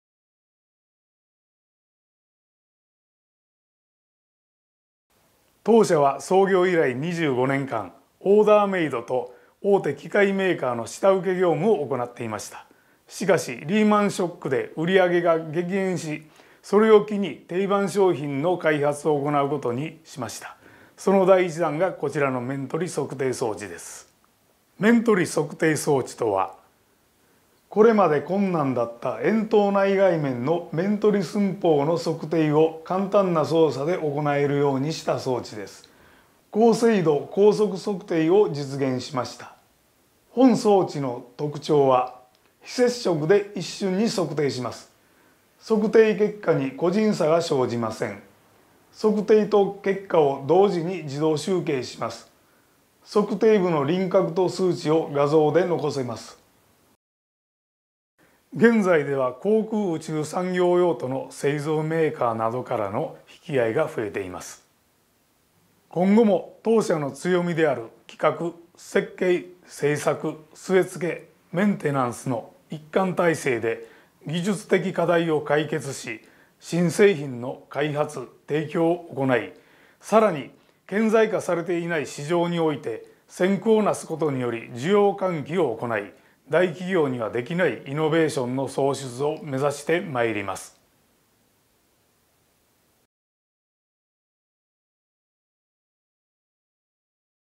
新価値創造NAVI－有限会社丸之内マシーナリ
新価値創造NAVI出展企業のご紹介です。有限会社丸之内マシーナリは、非接触で素早く正確に測定できる「面取り測定装置」を開発しました。